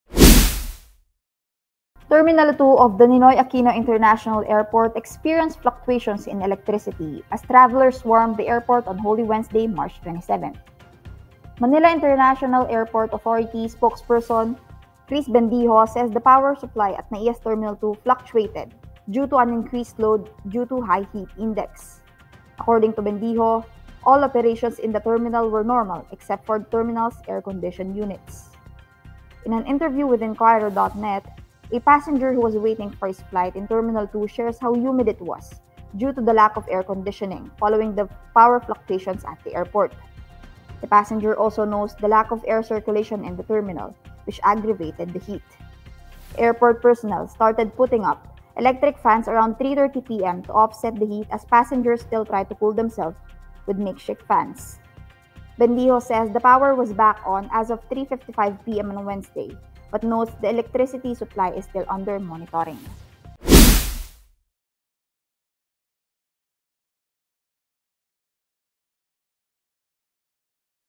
Electricity fluctuates at NAIA Terminal 2 on Holy Wednesday | INQToday
The Manila International Airport Authority says the Terminal 2 of the Ninoy Aquino International Airport experienced fluctuations in electricity as travelers swarmed the airport on Holy Wednesday, March 27.